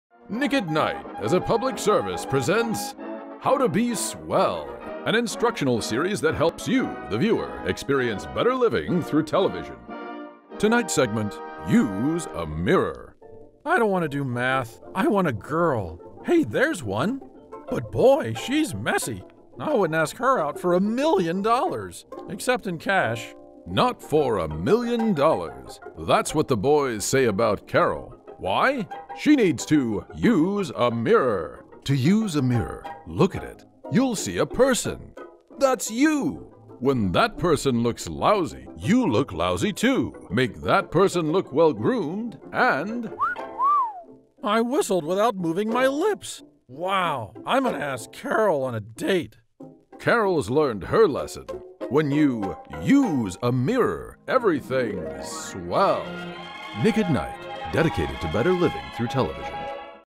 Use a Mirror | Classic How to be Swell
This is a remake of an episode of 'How to Be Swell', a sort of 'between commercials' entertainment shorts broadcast during the 'Golden Age' of Nick@Nite in the late 1980s to early 1990s, when the channel was at the height of its creativity and mild subversion. All of the voices featured in this video project were performed by me, including the Narrator and all the Duders.

A BIT ABOUT THE MAKING OF

Nick@Nite was created as a way to fill the evening programming after the kids' network Nickelodeon would go off-air in the evenings. They played 1950s and 60s black-and-white episodes of classic TV sitcoms like Donna Reed, Mr. Ed, Danny Thomas, Dobie Gillis and Patty Duke. Though a channel devoted to classic TV was novel at the time, what really made the station watchable were the commercials and shorts, many of which were ‘in-world’ facsimiles of retro TV with a whacky, sardonic edge. The introduction of later sketch comedy series like Laugh-In, the Smothers Brothers, SCTV, classic SNL and Fernwood 2 Nite really solidified this balance between nostalgia and subversion by injecting the innocent 50’s material with 60s and 70s counterculture edginess. It was entertaining, comforting, and strangely educational to see the pop-culture milieu of almost 3 decades of TV.

Especially shorts like ‘The Adventures of Milkman’ and faux-Public Service Announcements (PSAs) from ‘The President’s Council on Good TV’ assumed the straight-laced, nuclear-age aesthetic of the programming while also subverting it. 'How to Be Swell' was probably the best example of this, one-minute shorts produced by Nick@Nite to play during commercial breaks. They combed through old PSAs, mostly Coronet Instructional Films, to piece together these shorts with an authoritative, 50s-style narration, usually pushing something silly inherent in the original footage. Almost every short used mostly on PSA with other footage thrown in. This episode, ‘Use a Mirror', was mostly based on the Coronet film ‘Body Care’.

Alas, the brilliantly subversive Nick@Nite of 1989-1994 has given way to bland corporatized 80s & 90s re-runs and even blander commercials. You can find plenty of old N@N commercials, and even many of the shows they used to play, on YouTube.